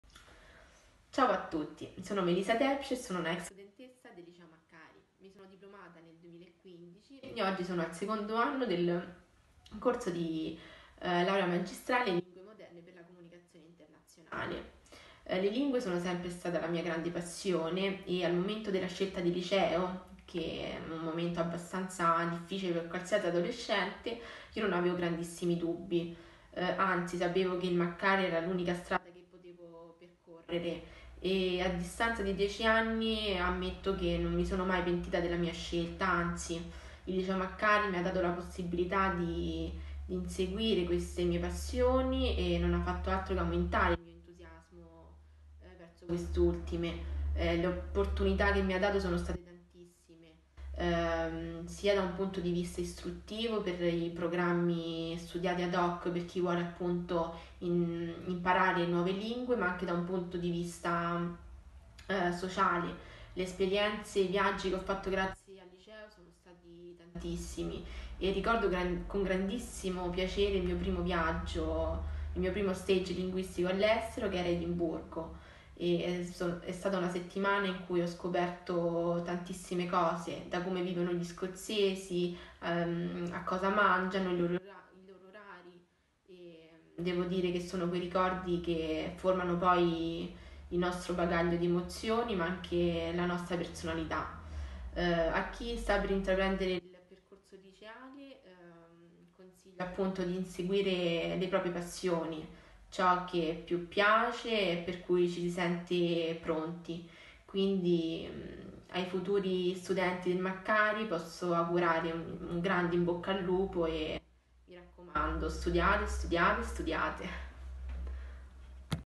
LICEO LINGUISTICO MACCARI: MELISSA
Liceo Maccari Testimonianza di Melissa ex alunna del Liceo Linguistico